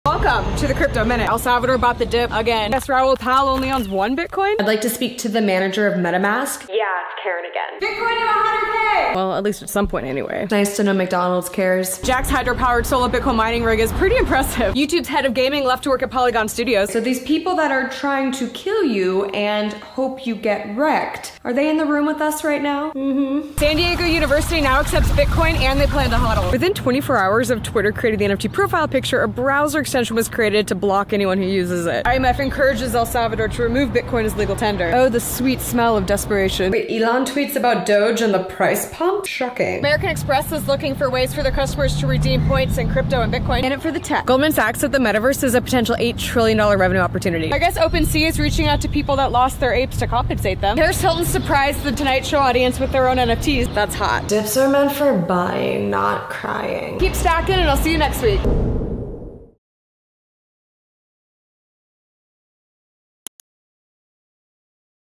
IMF urges El Salvador to drop BTC, Elon Musk tweet spikes DOGE price | The Crypto Minute
The Crypto Minute is your weekly crypto news rundown in just 60 seconds!